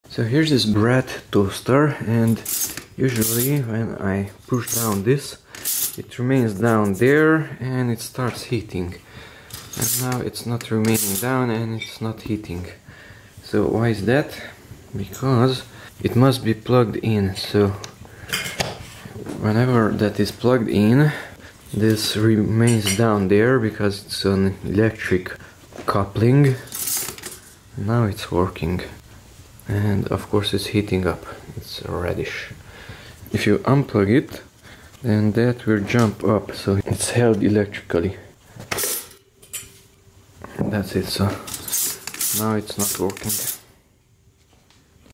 Why the Bread Toaster Lever Won't Stay Down
The lever is held down electrically. It remains in the down position only when it is powered.